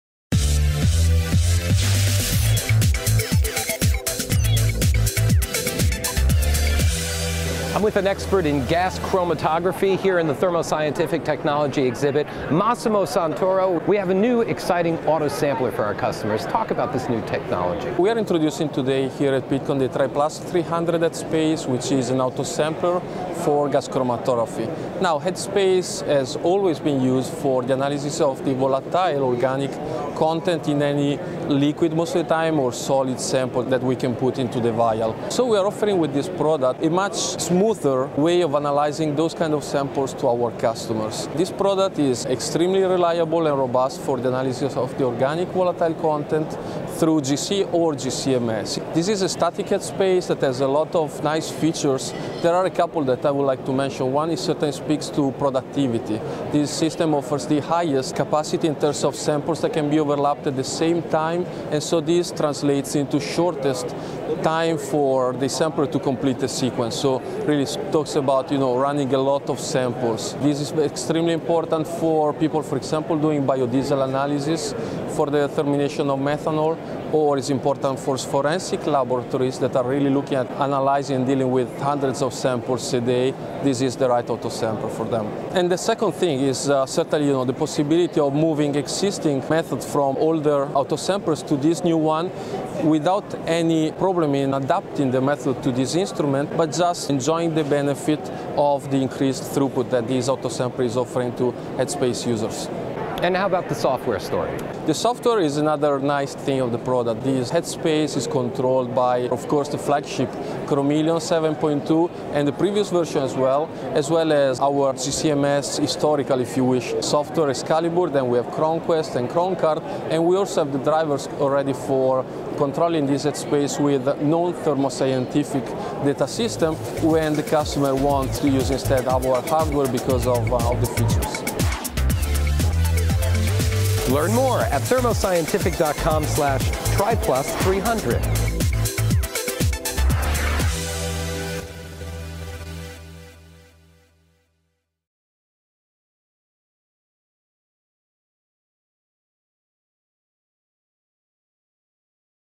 New Autosampler Designed for Fast Start-Up, High-Productivity
-- The new Thermo Scientific TriPlus 300 Headspace Autosampler is designed for fast start-up, high productivity and flexibility for analyzing organic volatiles. Throughput is enhanced by its 120-vial sample tray capacity and large 18-vial incubation oven overlap capacity. Global Gas Chromatography Product Manager Massimo Santoro, explains.